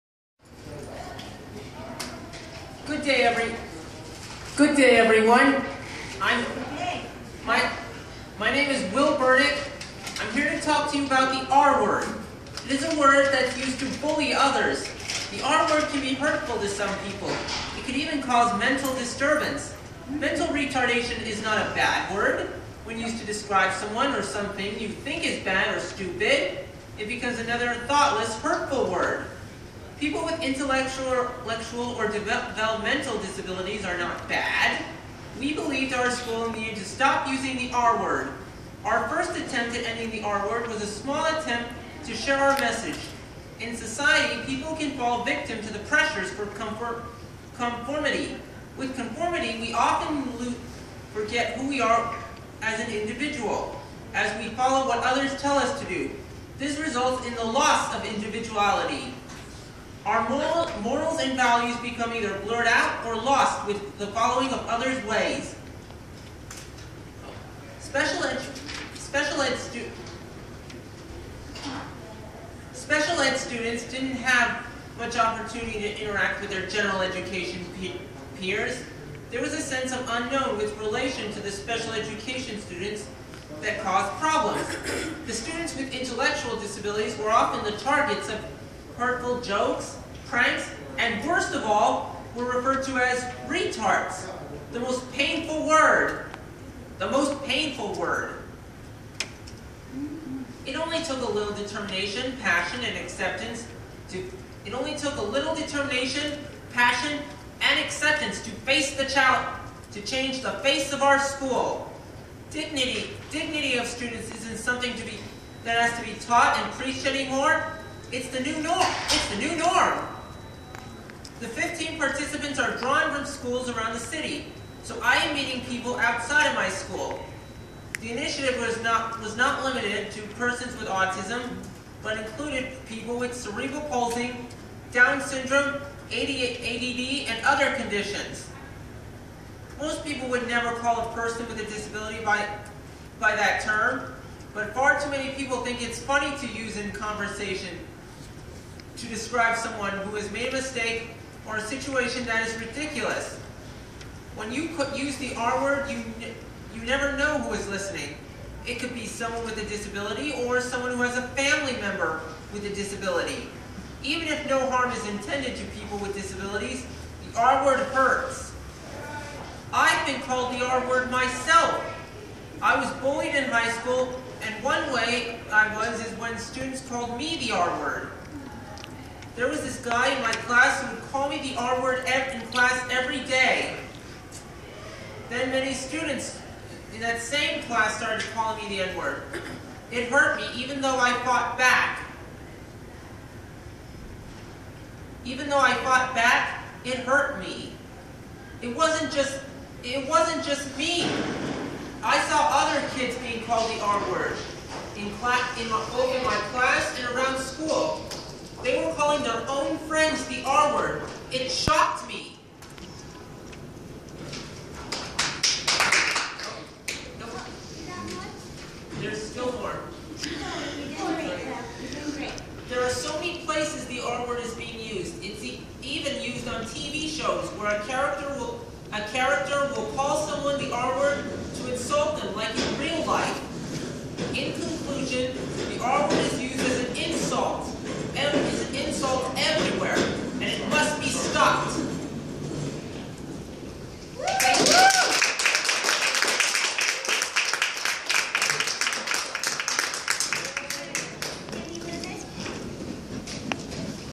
Will @ Spread the Word Clinic
USF hosted a Panel on how the R-Word is still affecting people even in 2015! Watch Will Bernick give his speech on ending the R-Word and what he has done to overcome the R-Word.